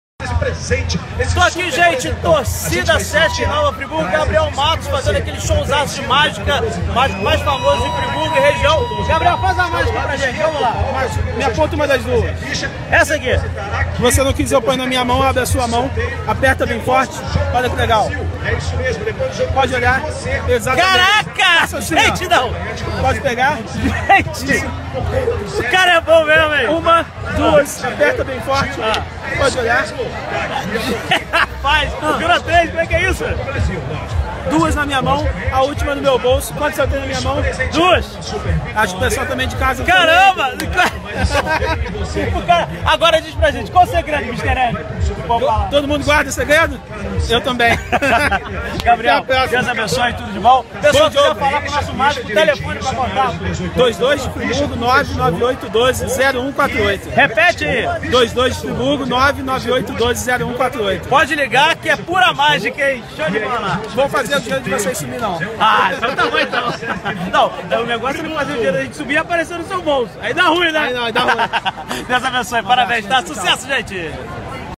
INCRÍVEL! MÁGICA NA RUA EM NOVA FRIBURGO! VEJAM SÓ!!!
TORCIDA PELO BRASIL EM
ANIMADA COM A MÁGICA DE GABRIEL MATTOS .